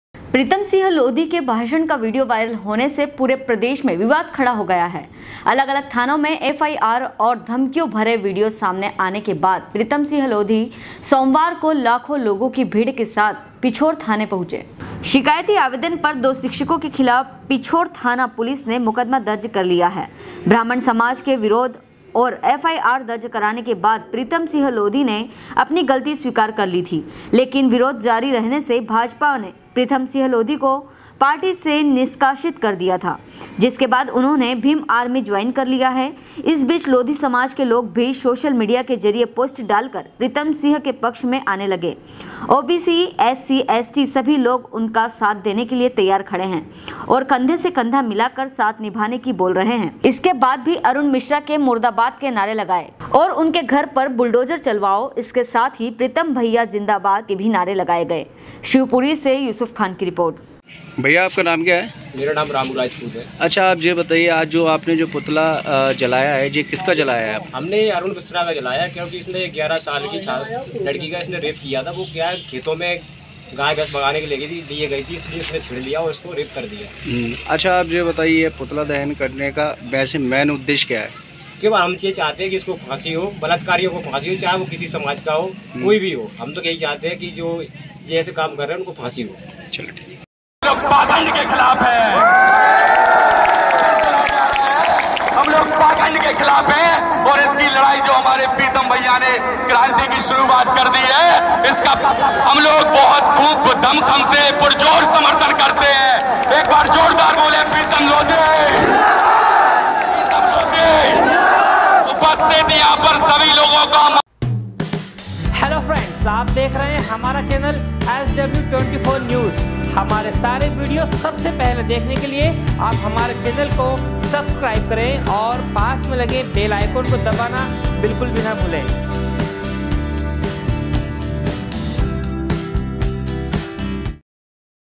प्रीतम सिंह लोधी के भाषण का वीडियो वायरल होने से पूरे प्रदेश में विवाद खड़ा हो गया गया है।
प्रीतम सिंह लोधी के भाषण का वीडियो वायरल होने से पूरे प्रदेश में विवाद खड़ा हो गया गया है। अलग-अलग थानों में एफआईआर और धमकियों भरे वीडियो सामने आने के बाद प्रीतम सिंह लोधी सोमवार को लाखो लोगो की भीड़ के साथ पिछोर थाने पहुंचे। शिवपुरी से युसूफ खान की रिपोर्ट